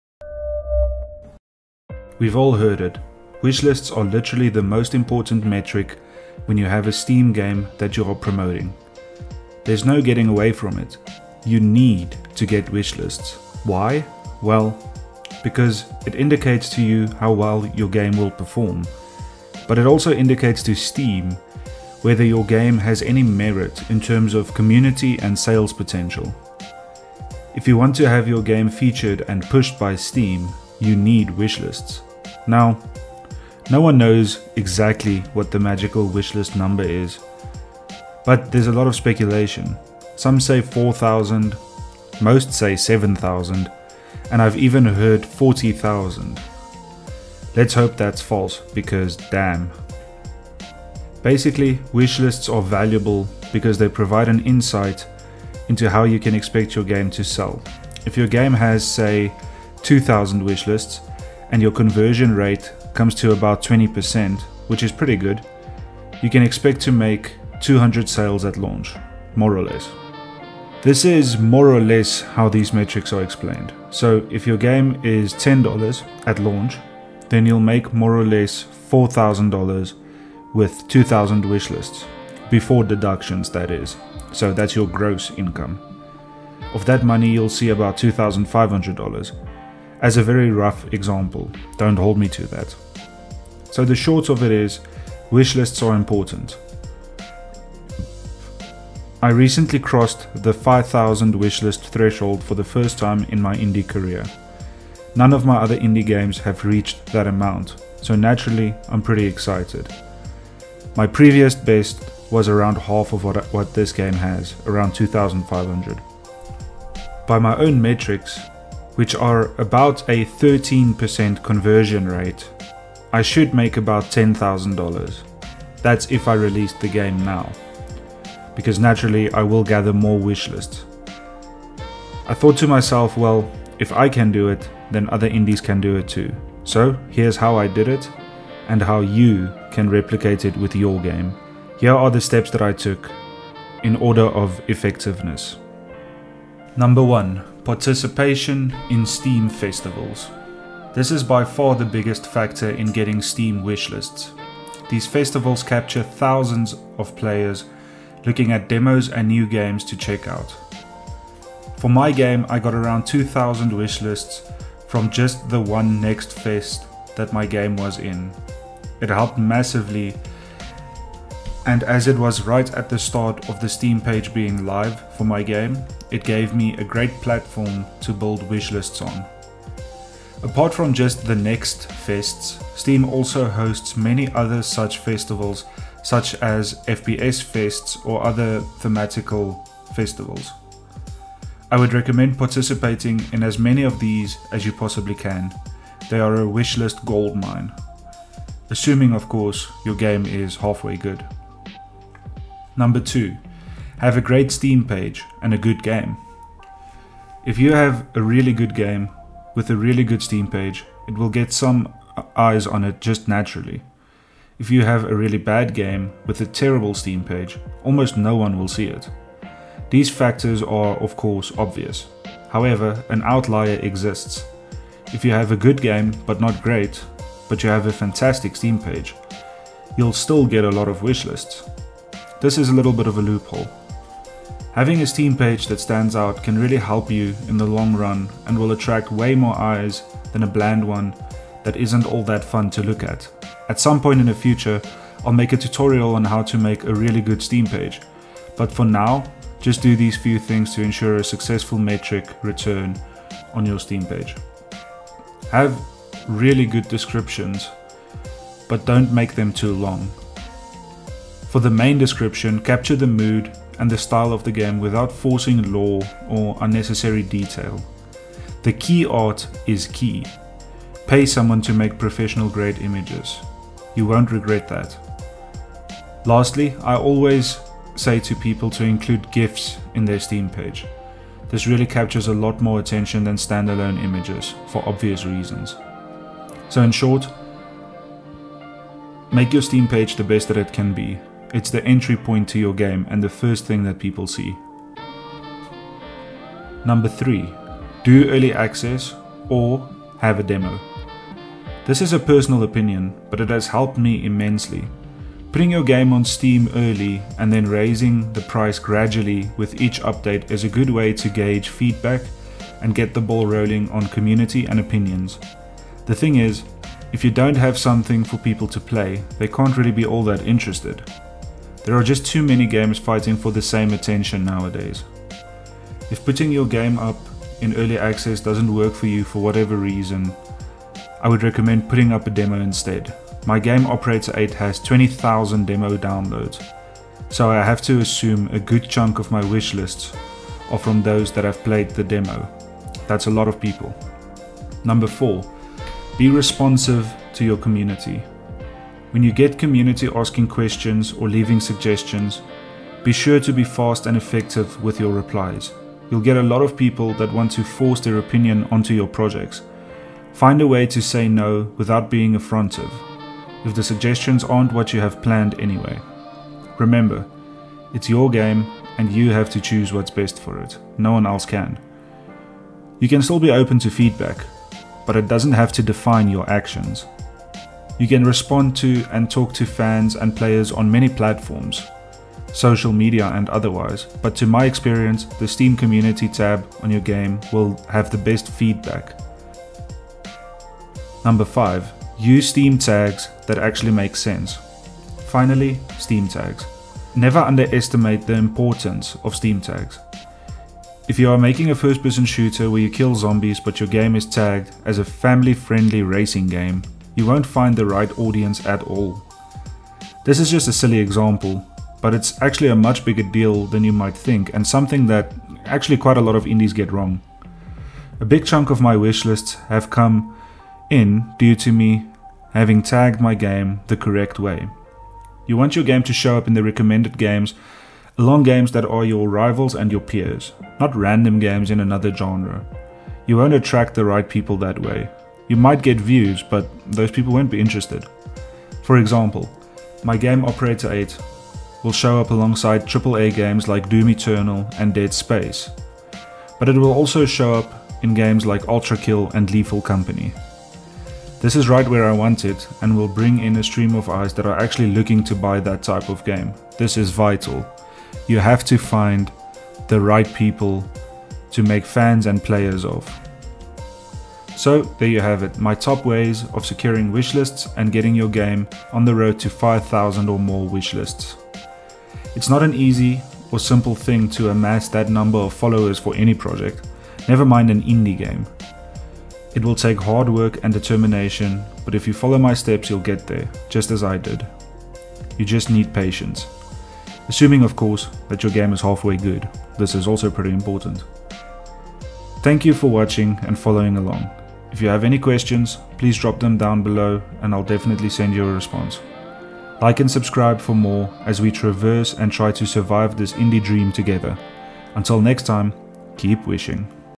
How To Get 5000 Wishlists On Steam...
Making games is hard, we all know this. What's even harder though, is selling them. Getting people to notice and be interested in your games is one of the most difficult undertakings you can attempt!

This is how I got 5000 people to wishlist my game, and how you can too!